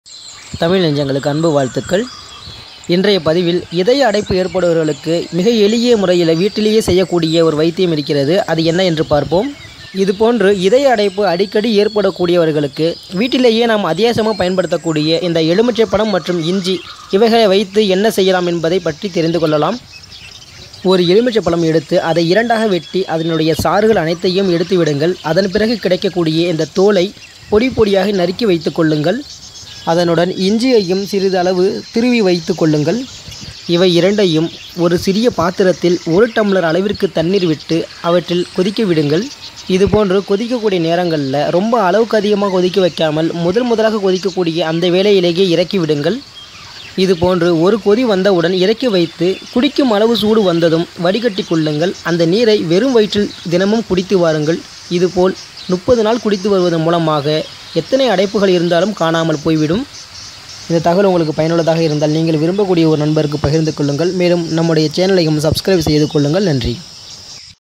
இதய அடைப்பு இருக்கா?அப்ப இதை மட்டும் செய்து பாருங்கள்|Home Remedies
அடிக்கடி இதய அடைப்பு ஏற்படுகிறவர்களுக்கான வீட்டு வைத்தியம் தரப்பட்டுள்ளது...

பிடித்திருந்தால் நீங்கள் விரும்பக்கூடிய ஒரு நண்பருக்கு பகிருங்கள்(Share)

"தெரியாதவற்றை தெரிந்துக்கொள்வோம்
தெரிந்தவற்றை பகிர்ந்துக்கொள்வோம்"